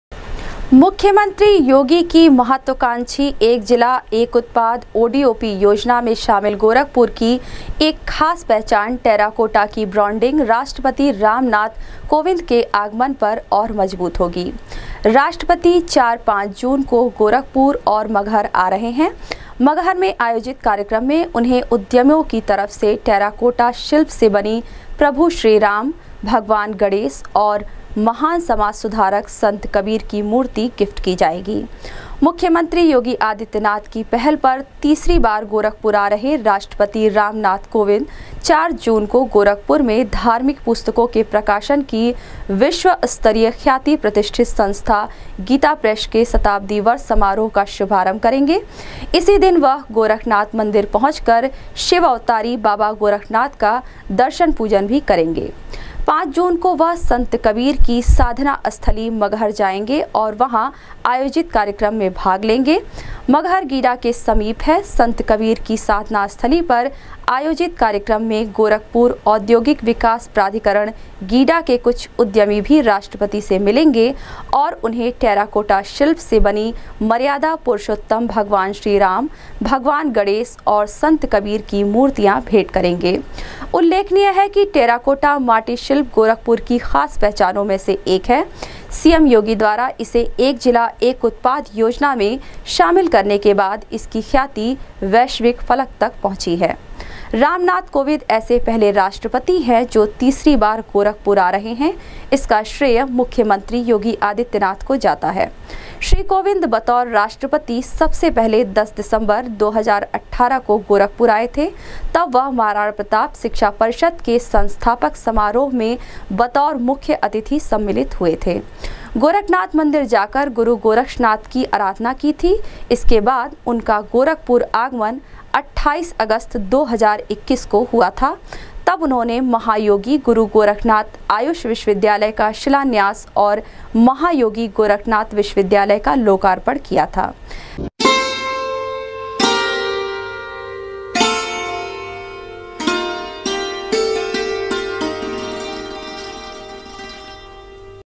राष्ट्रपति रामनाथ कोविंद को गिफ्ट करेंगे टेराकोटा के श्रीराम और कबीर, ओडीओपी में शामिल टेराकोटा की
राष्ट्रपति रामनाथ कोविंद को गिफ्ट करेंगे टेराकोटा के श्रीराम और कबीर, ओडीओपी में शामिल टेराकोटा की और मजबूत होगी ब्रांडिंग,सीएम योगी की पहल पर तीसरी बार गोरखपुर आ रहे हैं राष्ट्रपति,चार जून को गीता प्रेस और पांच जून को मगहर में है महामहिम राष्ट्रपति का कार्यक्रम